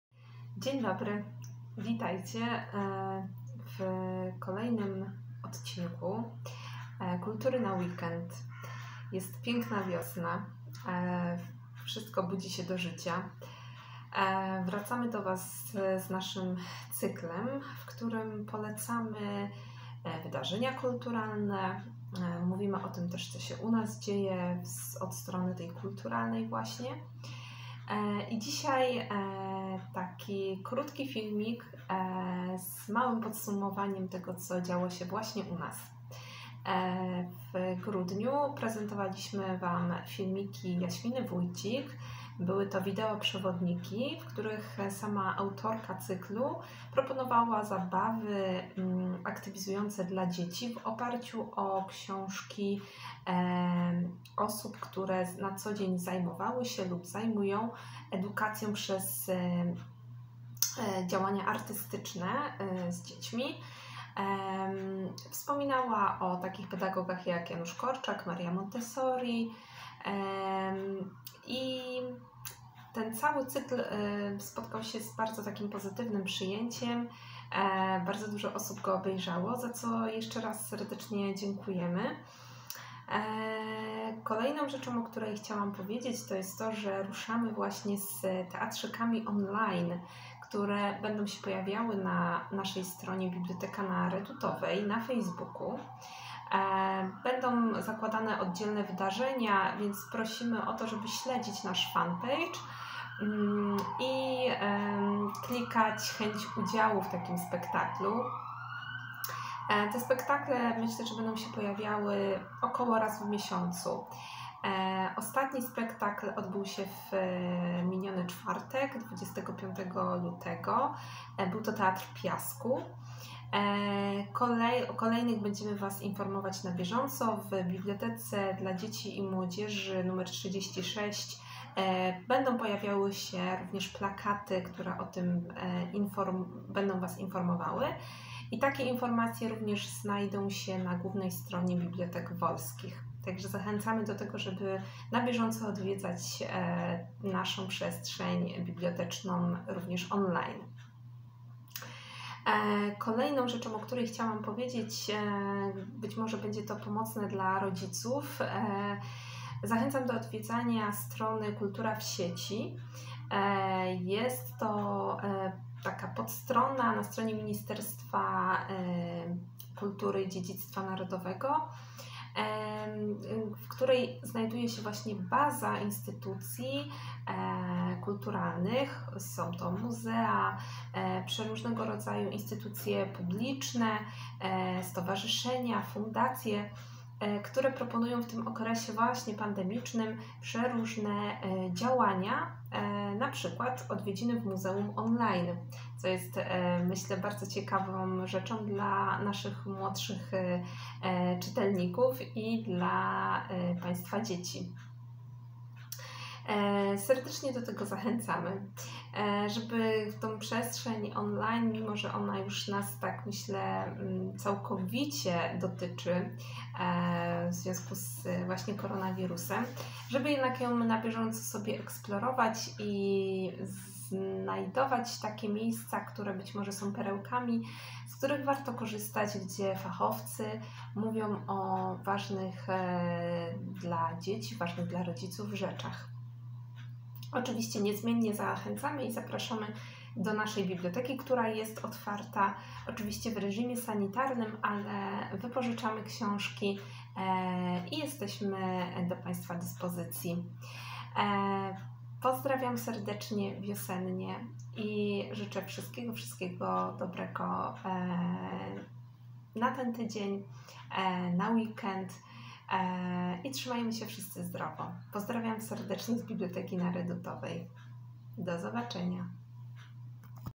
KulturaNaWeekend - Co u nas słychać? 👂
Po dłuższej przerwie  zapraszamy do posłuchania, co ciekawego działo się u nas w Bibliotece przez ostatni czas... oczywiście od strony kulturalnej 🙂

Zapraszamy do odwiedzin w Bibliotece oraz na naszej stronie na FB: Biblioteka na Redutowej. 🥰